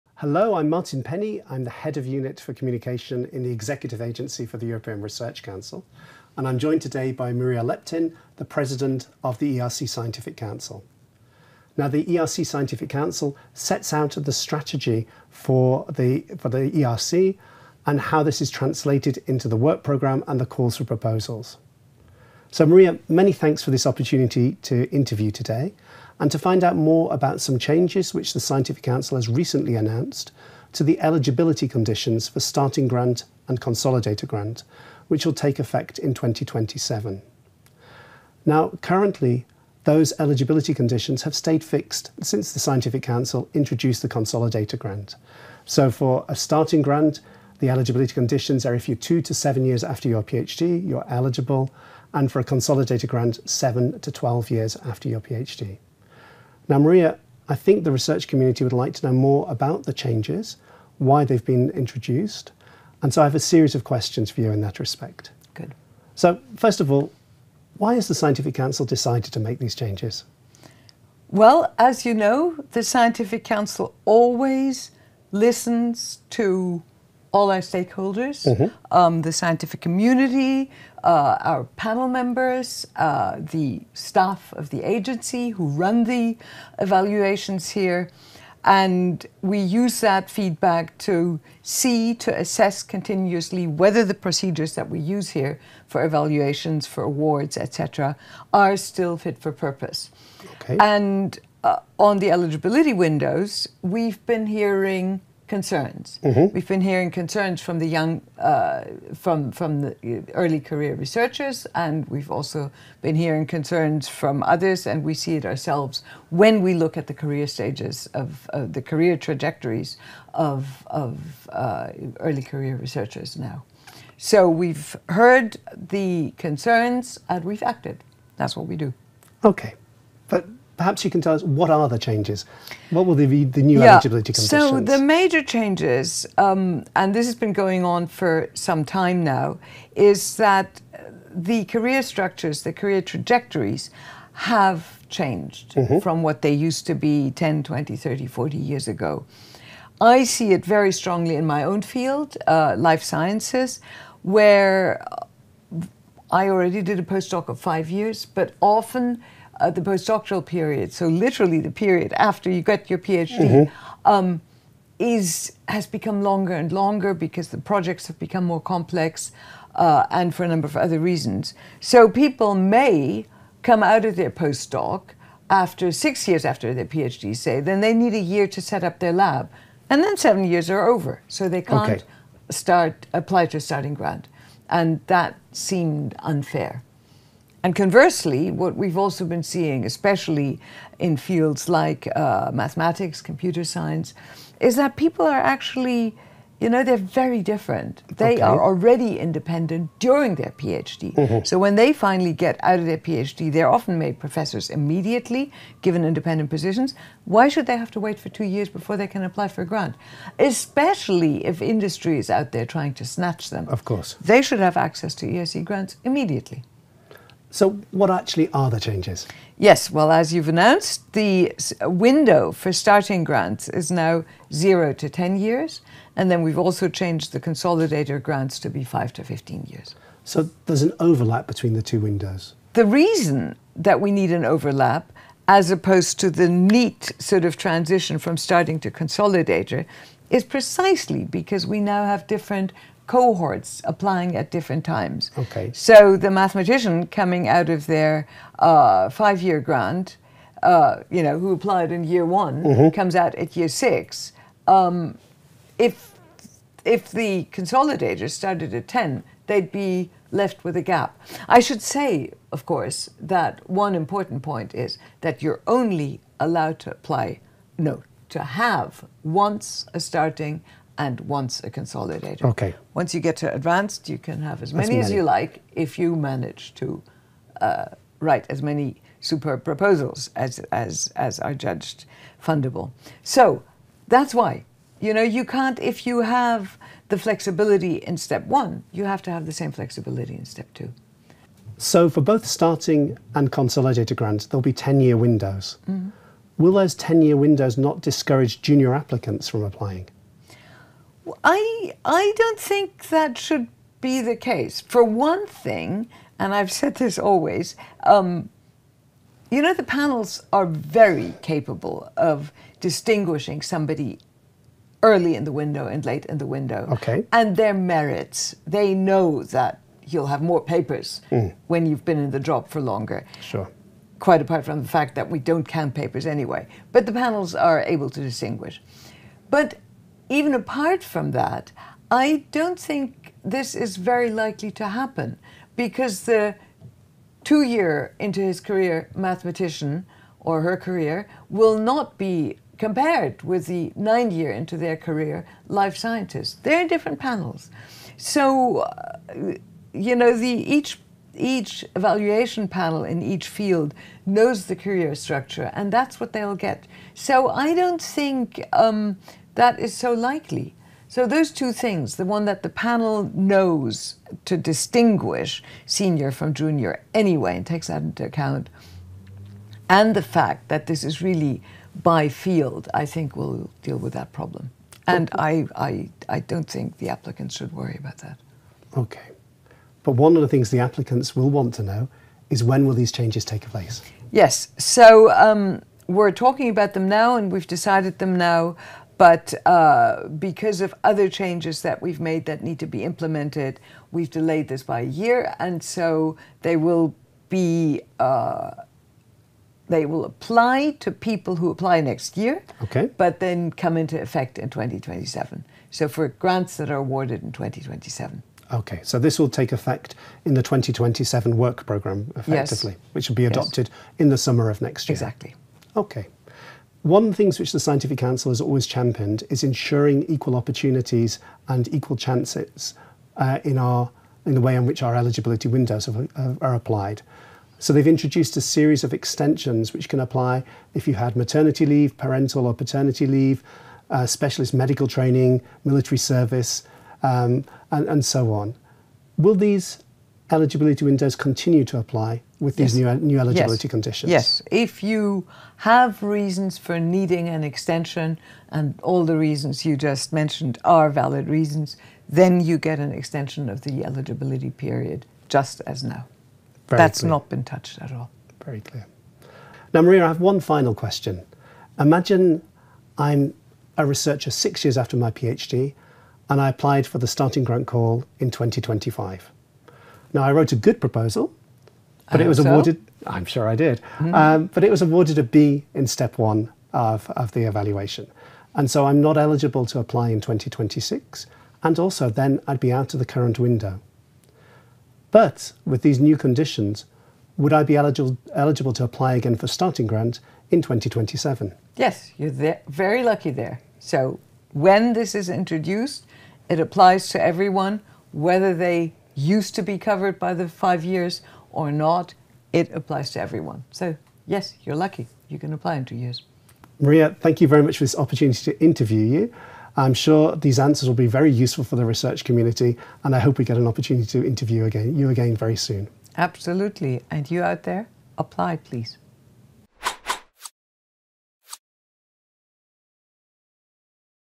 What’s changing in ERC eligibility for 2027? Maria Leptin explains
From 2027, the ERC will extend the eligibility windows for its Starting and Consolidator Grants to better reflect the diverse career paths of researchers across Europe.
Starting Grants will be open from PhD defence up to 10 years, and Consolidator Grants from 5 to 15 years post-PhD.
ERC President Maria Leptin explains how these adjustments increase flexibility and fairness across disciplines.